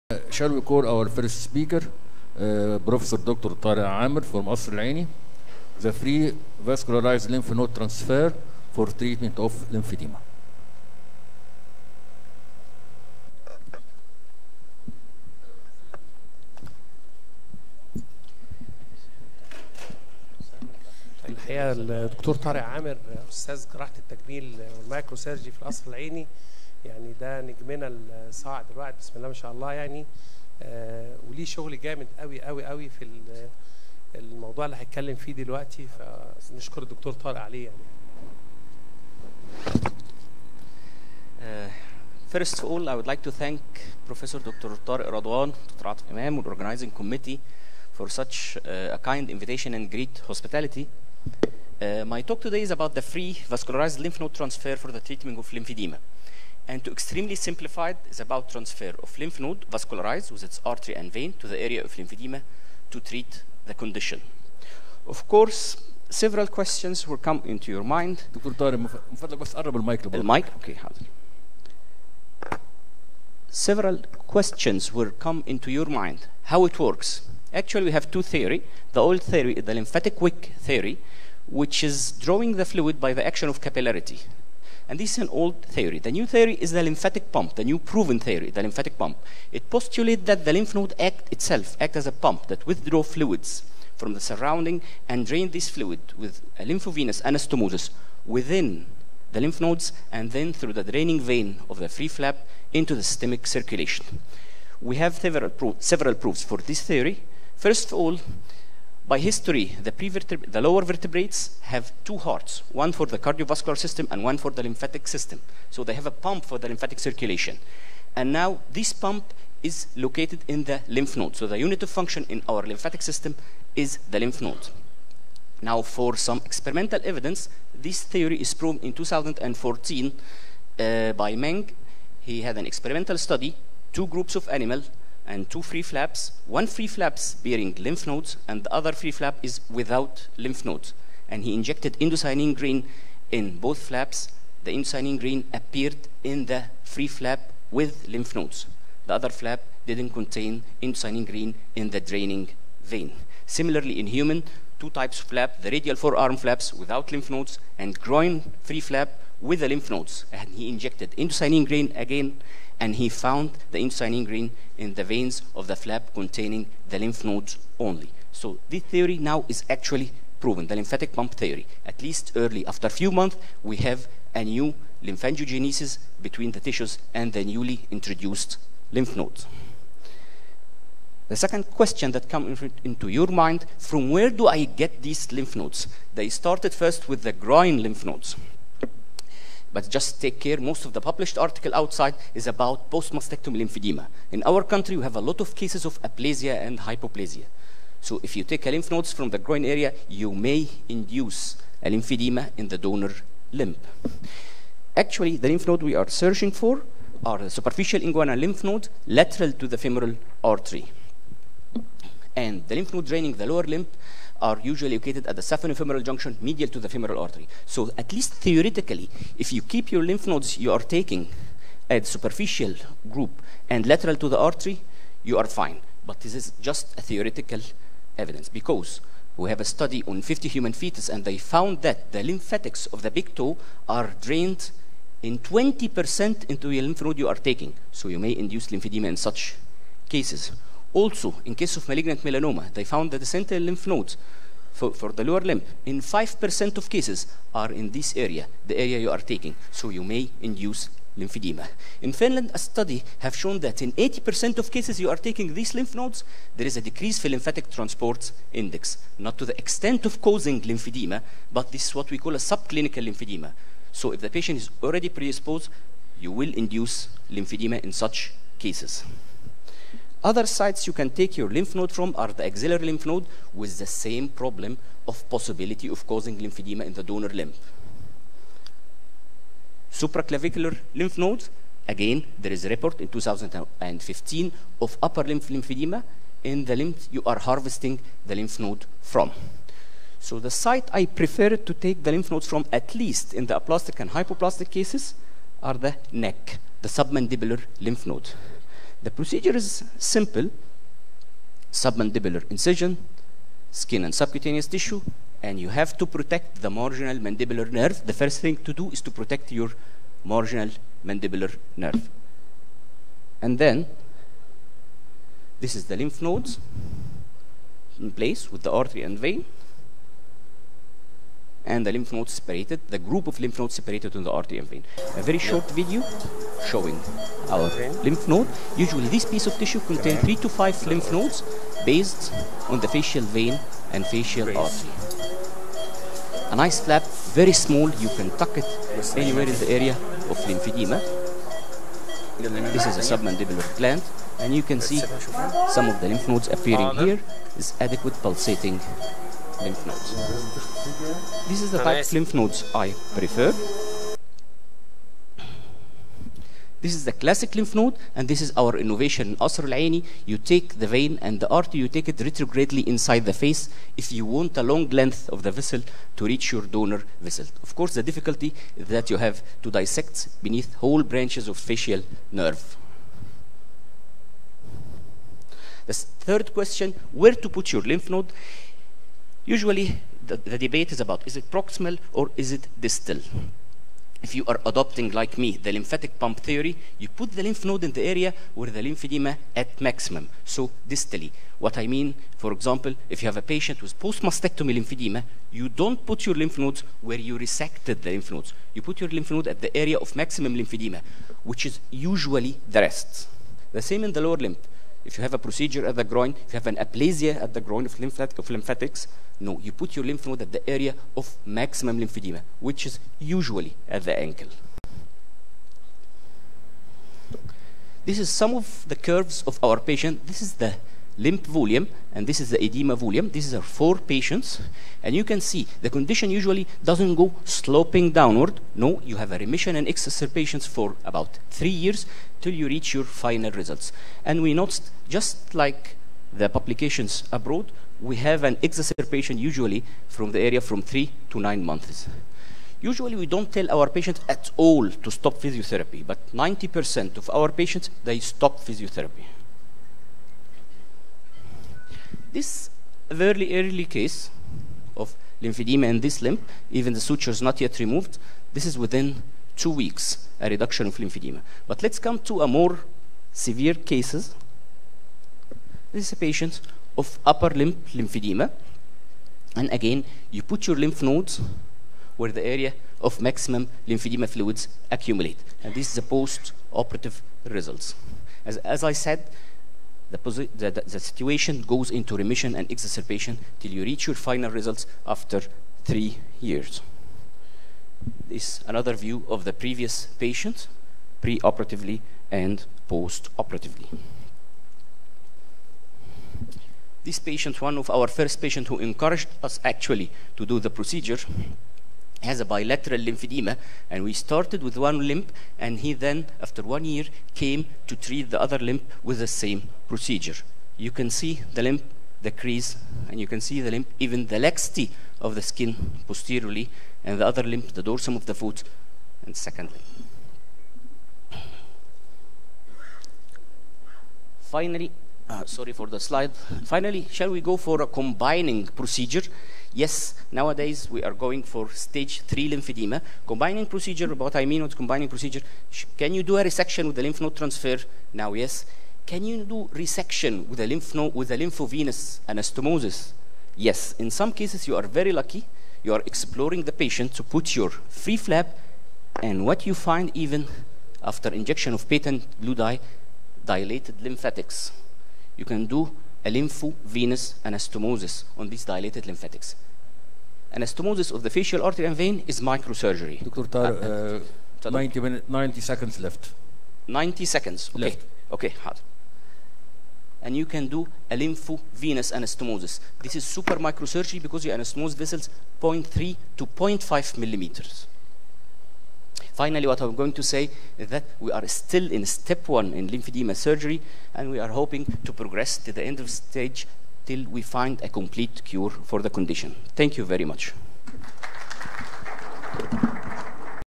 Vascular online training EVC14th Prof Tarek amer lecture on lymphnode transfer
vascular online training very attractive technique waiting for clinical evaluation to reach clinical practice